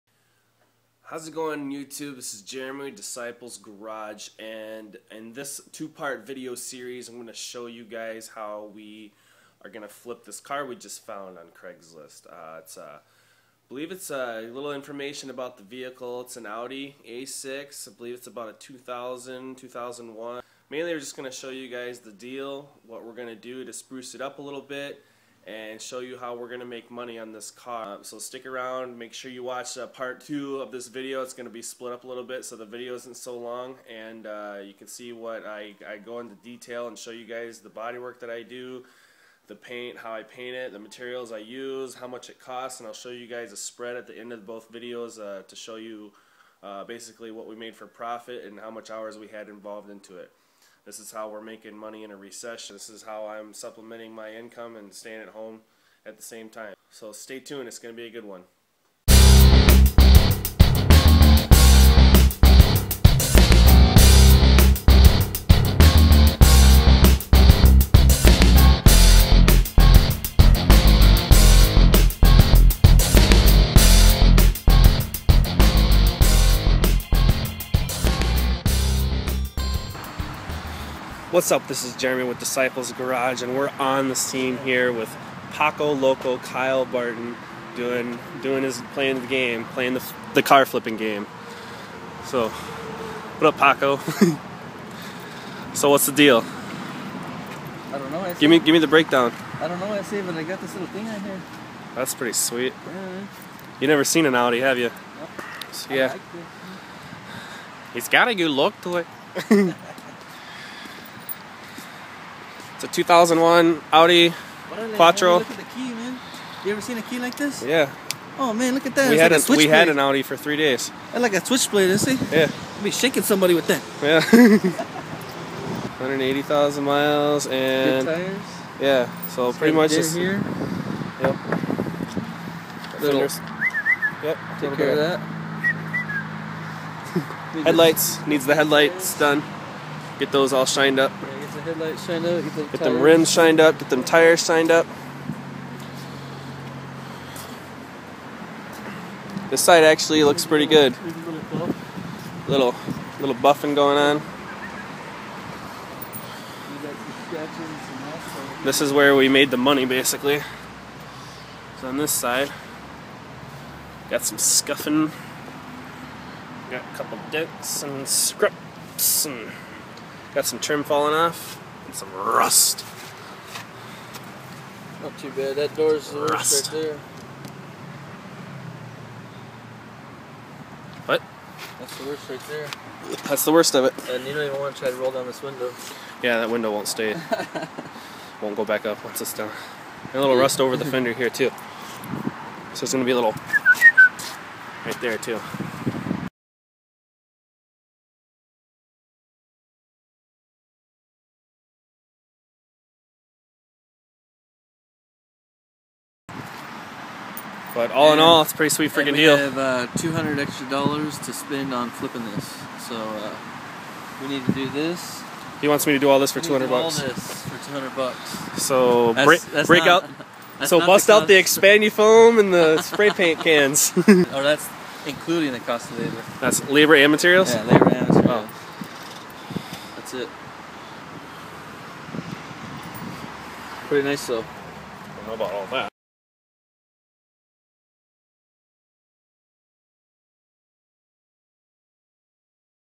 How to Flip Cars on Craigslist - 01 Audi A6 Quattro (finding a car to flip)
Youtube.com/DeciplesGarage shows you how to flip cars on craigslist. We're flipping cars for profit this week with this nice Audi A6 Quattro. It need a little work but we will show you the work needed to fix and flip for quick cash. Buying and selling used cars is a big market right now and you could make money from home flipping cars.